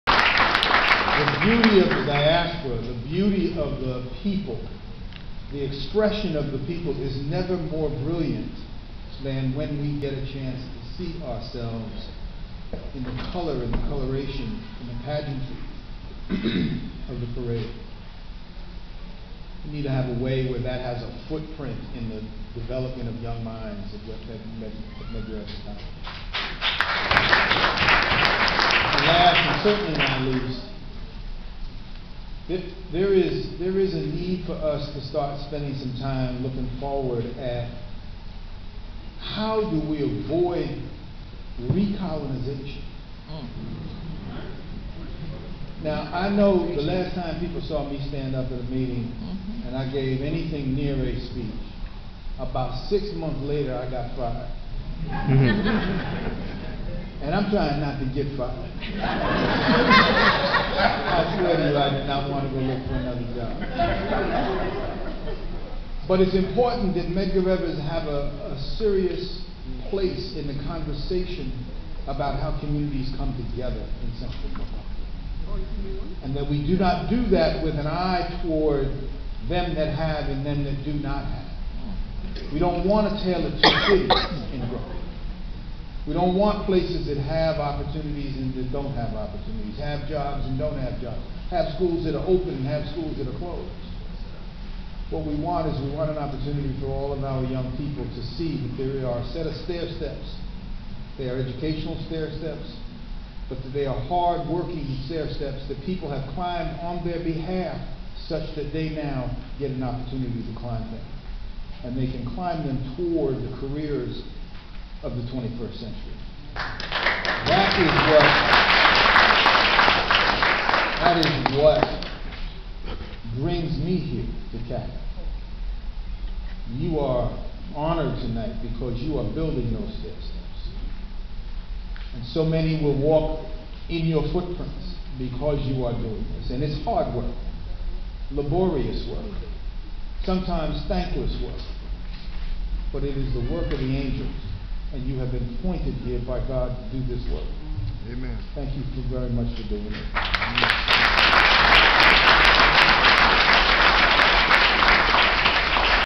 Dr Rudy Crew, Medgar Evers College president speaks to CACCI
Dr. Rudy Crew, president of Medgar Evers College and a former NYC schools chancellor told an audience of Caribbean American business leaders that his college must play a role in community and youth development.